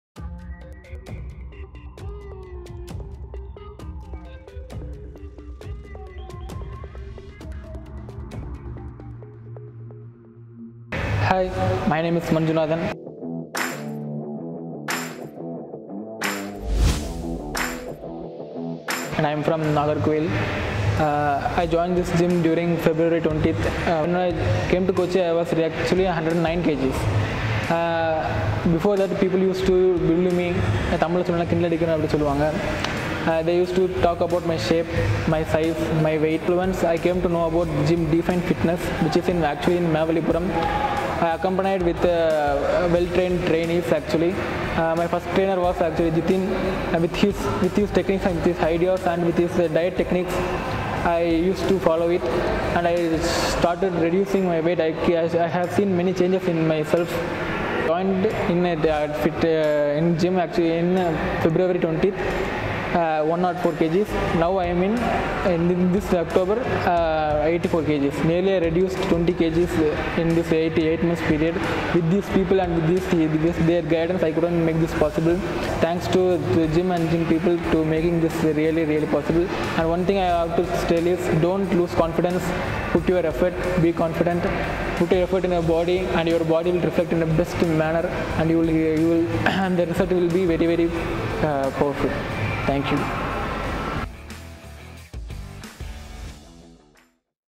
Transformation Story & Workout Motivation at Best gym in Kakkanad, Kochi
Watch out our Member Manchunathan's Experience at D5NFitness, He reduced around 20 Kg within few months & We are very happy to see your Transformation.. Keep going...